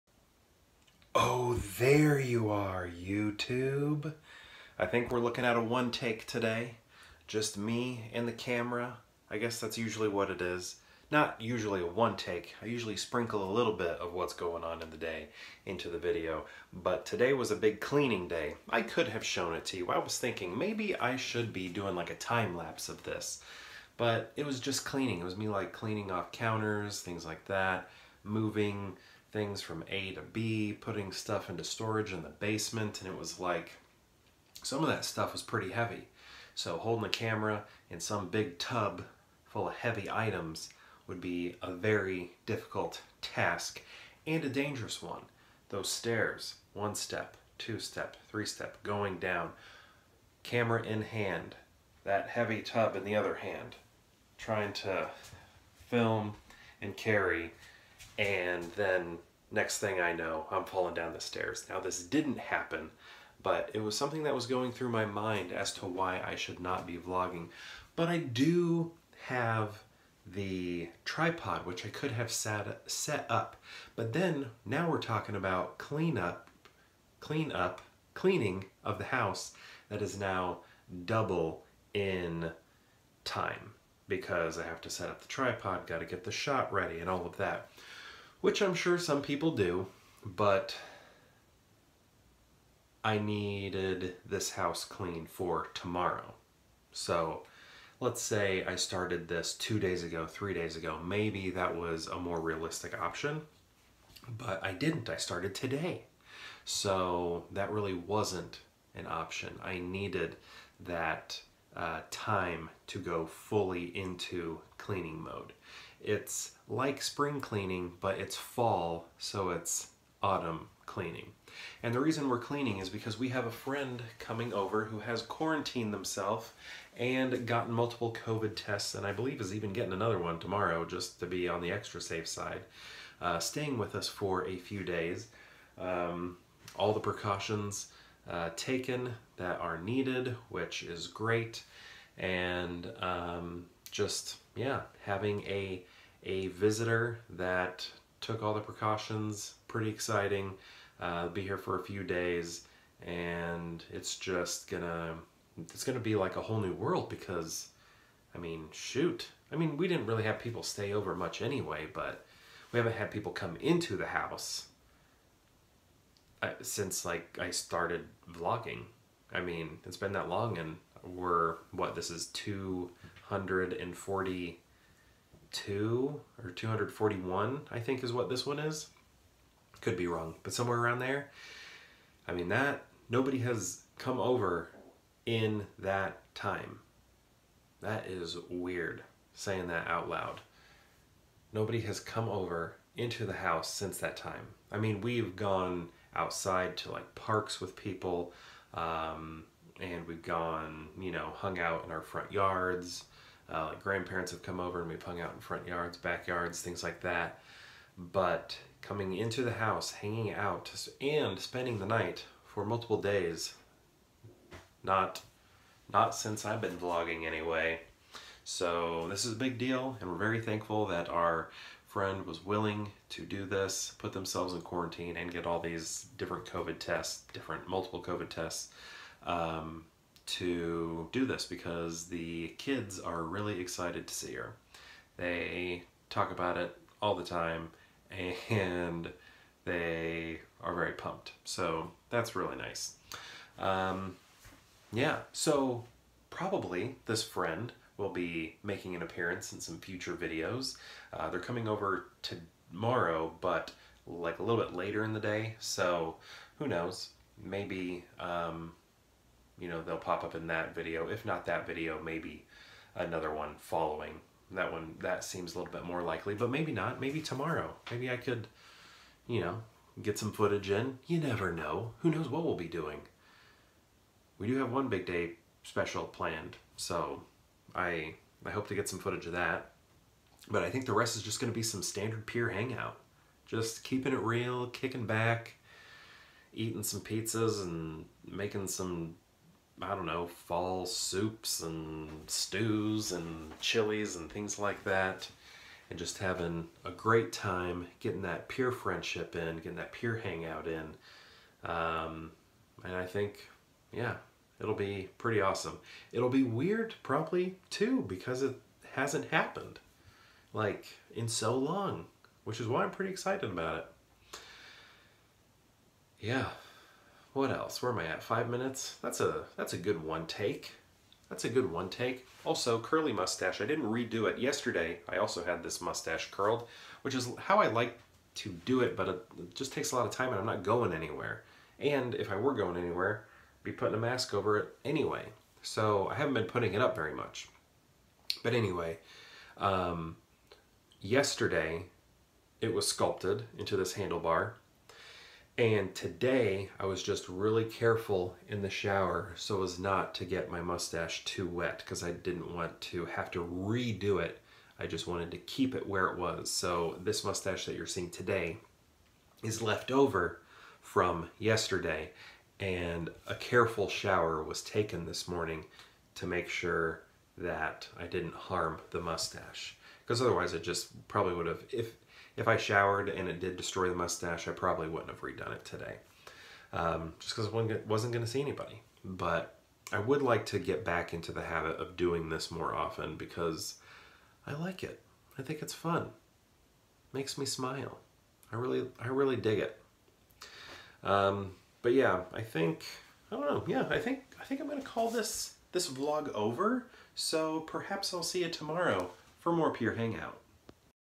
A story about cleaning the house in preparation for a visitor who will be joining us tomorrow | V240
A story about cleaning the house in preparation for a visitor who will be joining us tomorrow.

I also talk about my handlebar mustache a bit.